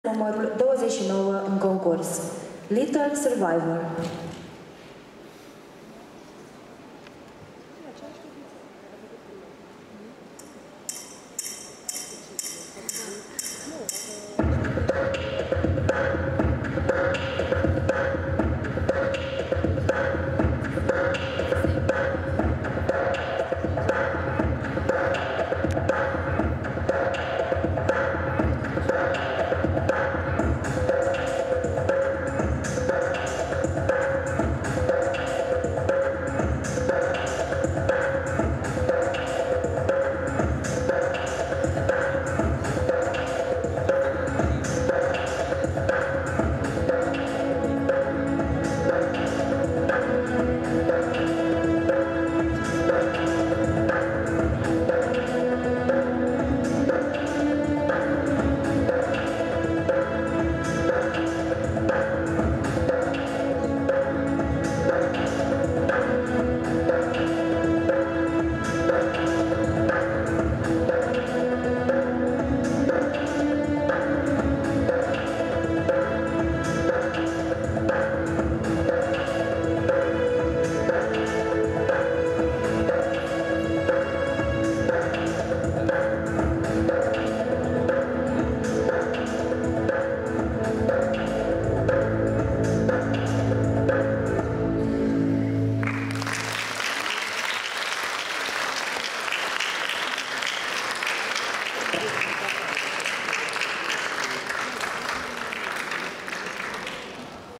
2016 Attitude Ballet Festival
Attitude Ballet Festival este un proiect al Asociatiei culturale “Reverence”, Colegiului National de Arta “George Apostu” si Teatrului Municipal Bacovia, care angreneaza tinerii din invatamantul de stat si privat intr-un festival dedicat artei dansului. Festivalul este o promovare a limbajului coregrafic in spatiul cultural bacauan, care cuprinde competitia “Attitude Ballet Competition”, Gala de Balet, Parada costumelor de balet, workshopuri de dans clasic si dans contemporan, cursuri de vara.